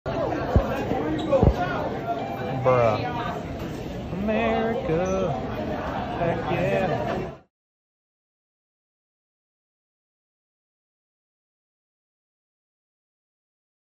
Traveler Brings Bald Eagle Through TSA in North Carolina
THE EAGLE HAS LANDED: A traveler took what appeared to be a bald eagle through airport security in Charlotte, NC, where it showed off its large wings. According to local news outlets, TSA was aware ‘Clark the Eagle’ would be arriving for screening.